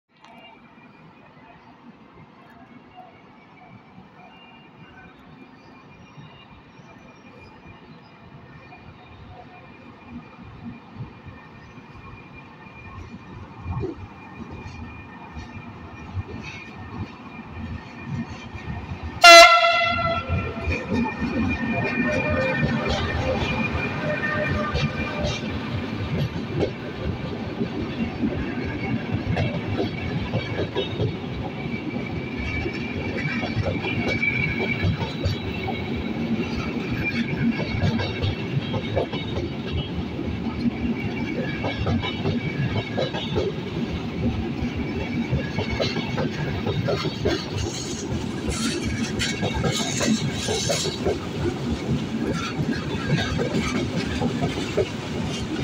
Intercitynotte in partenza da Torino Porta Nuova (3 carrozze Nuova livrea in coda)
Video fatto da Erik